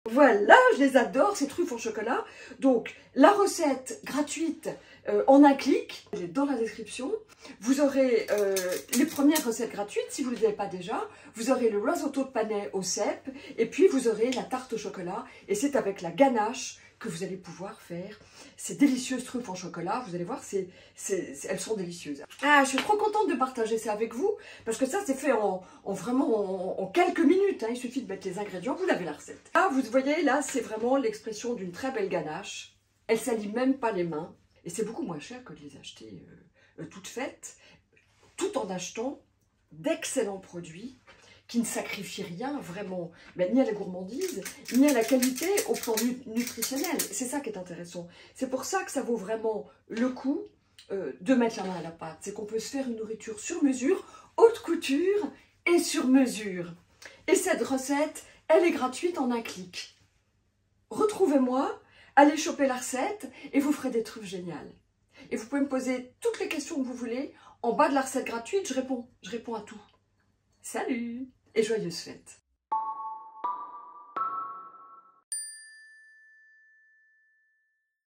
Mon cadeau pour les fêtes : une recette de truffes en chocolat !
Mon cadeau pour les fêtes, une recette de truffes en chocolat ! pour recevoir la recette gratuite de la Tarte au Chocolat, si vous ne l'avez pas déjà, vous recevrez en même temps la recette festive du Rawsotto® crémeux de Panais aux Cèpes! La recette de la Ganache de la Tarte au Chocolat est la même que celle des truffes. Il vous suffit de remplacer 1/3 de l'huile de coco par 1/3 de beurre de cacao... et vous pourrez rouler des truffes exceptionnelles, pour satisfaire les plus fins gourmets et les gourmands soucieux de p^réserver leur vitalité avec des produits excellents et guilt free !
Retrouvez-moi en un clic, je vous attends !
Joyeusement !
Votre NatuRawChef®,
Marie-Sophie